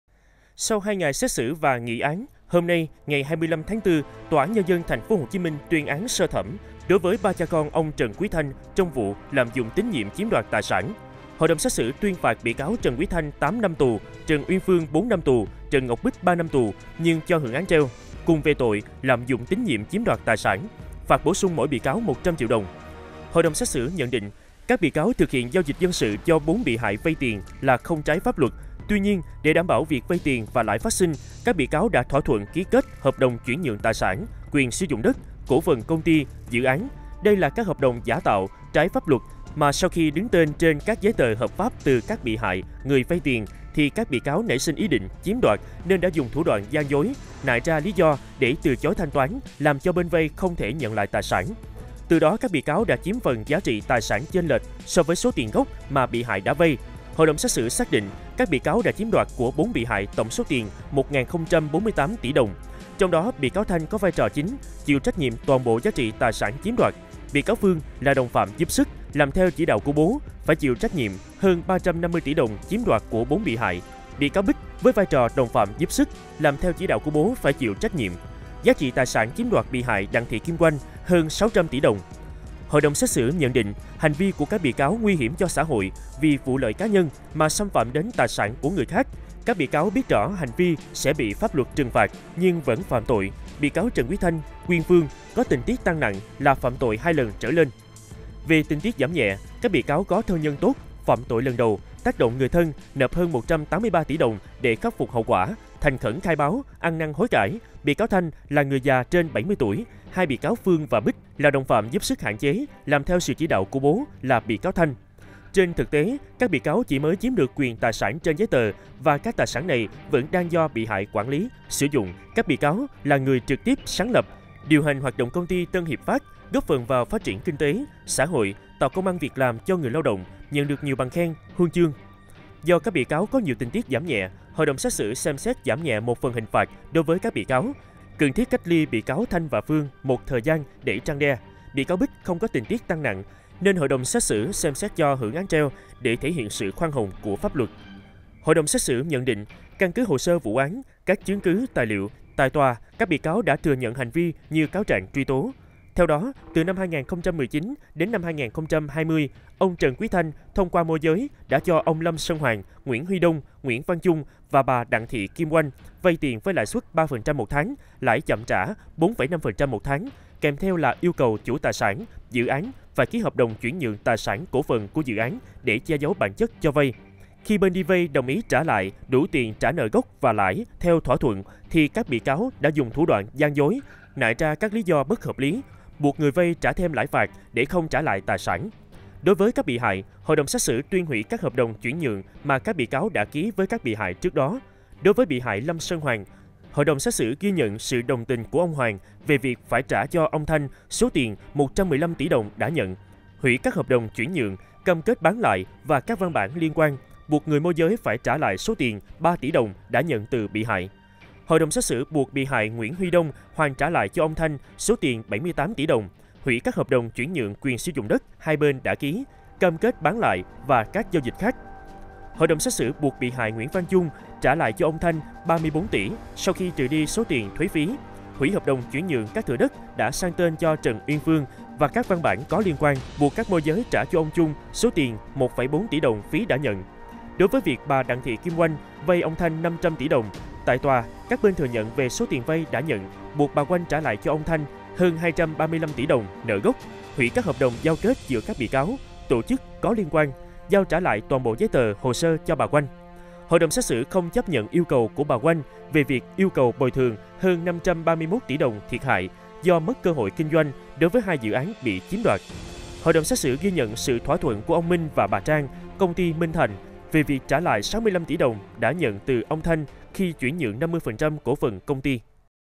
NÓNG: Tuyên phạt ông Trần Quí Thanh 8 NĂM TÙ | CafeLand
Sau hai ngày xét xử và nghị án, hôm nay (ngày 25-4), TAND TP.HCM tuyên án sơ thẩm đối với ba cha con ông Trần Quí Thanh trong vụ lạm dụng tín nhiệm chiếm đoạt tài sản.
HĐXX tuyên phạt bị cáo Trần Quí Thanh 8 năm tù, Trần Uyên Phương 4 năm tù, Trần Ngọc Bích 3 năm tù nhưng cho hưởng án treo, cùng về tội lạm dụng tín nhiệm chiếm đoạt tài sản. Phạt bổ sung mỗi bị cáo 100 triệu đồng.